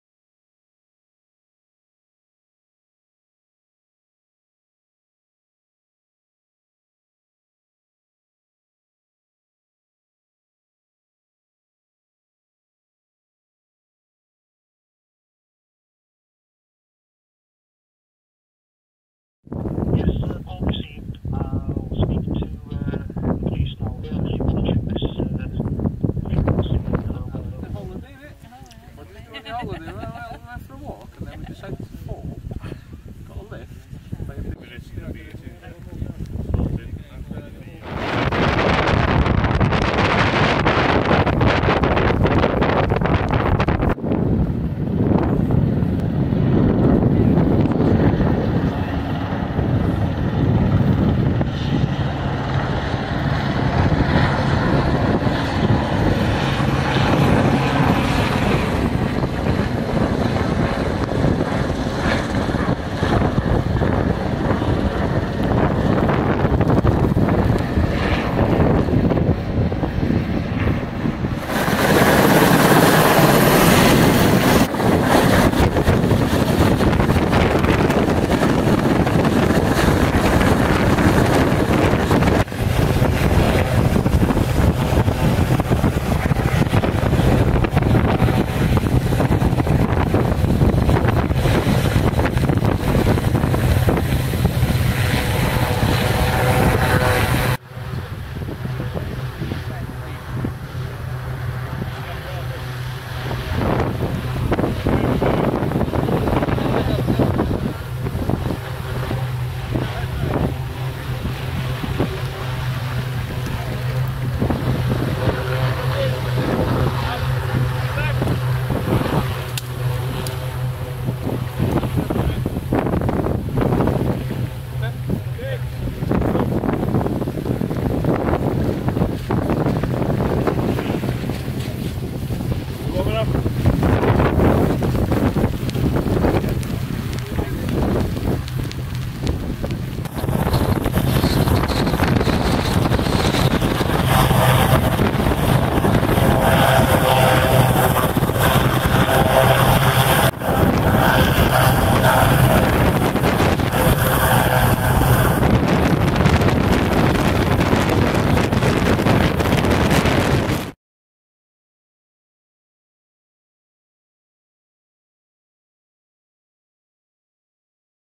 Pen y Gaer Rescue - Aberglaslyn Mountain Rescue Team
Rescue of walker from Pen y Gaer, near Aberglaslyn Pass, Beddgelert. The casualty had slipped and damaged her knee and could not walk.
SAR Helicopter from Valley and South Snowdonia SAR Team also attended.